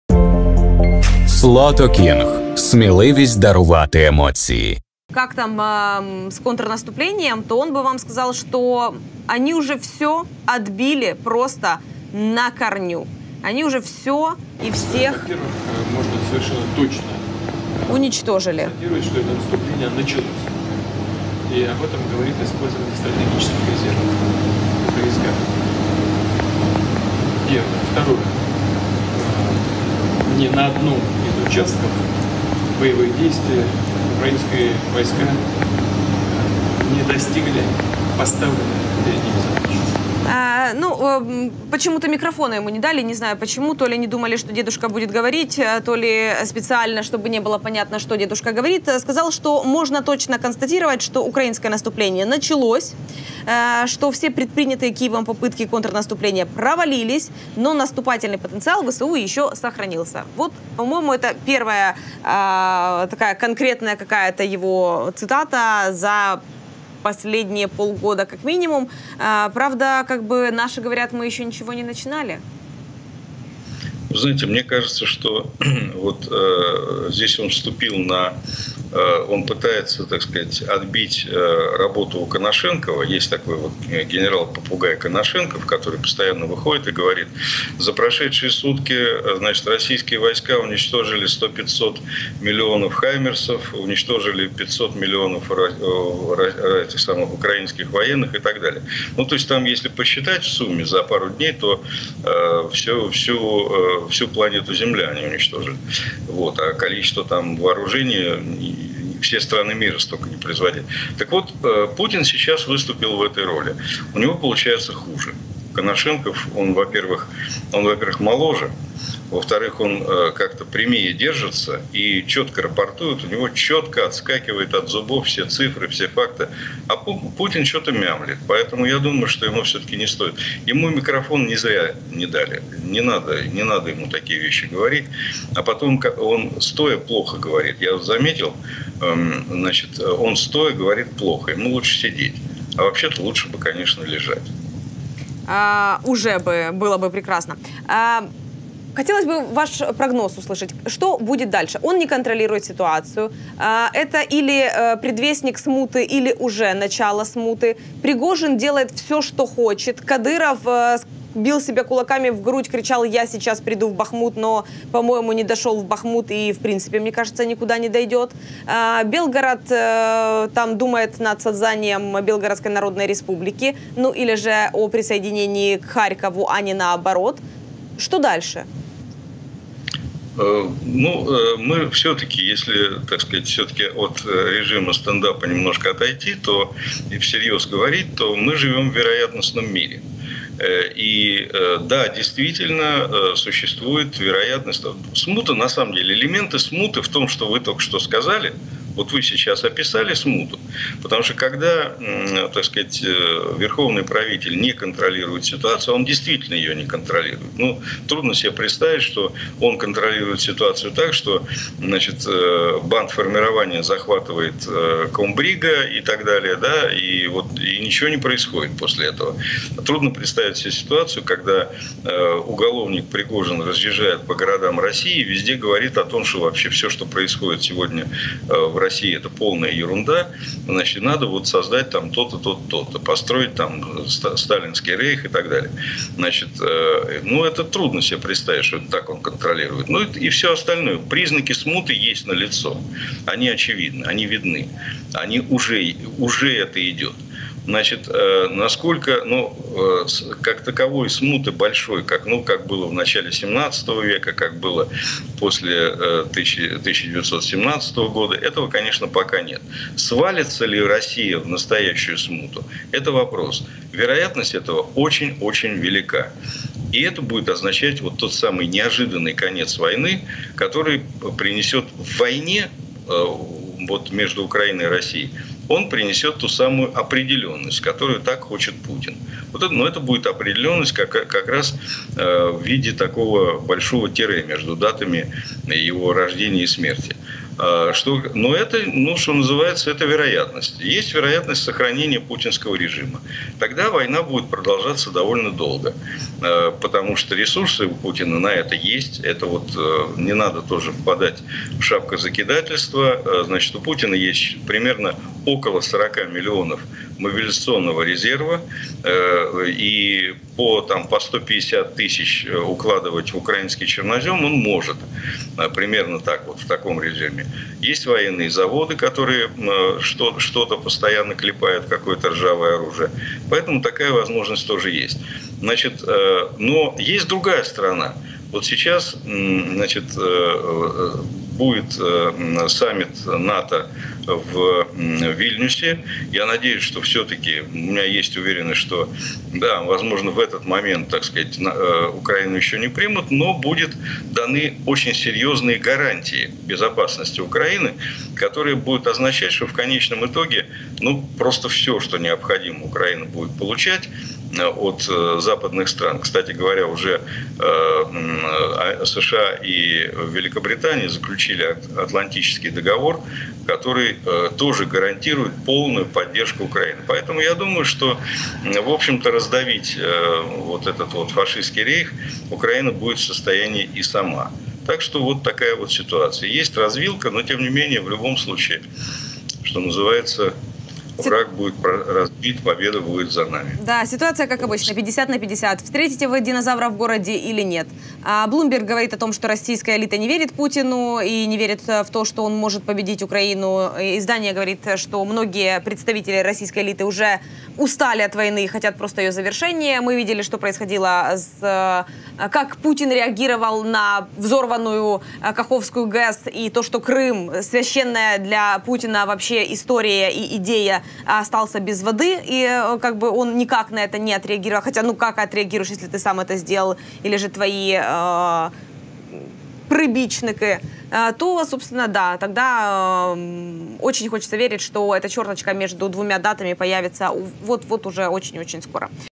ЯКОВЕНКО: Заява Путіна про контрнаступ / Бункерний зганьбився
Журналіст Ігор Яковенко @IgorYakovenko  висловився про заяву Путіна про контрнаступ ЗСУ. Також він висловився щодо ситуації всередині Росії.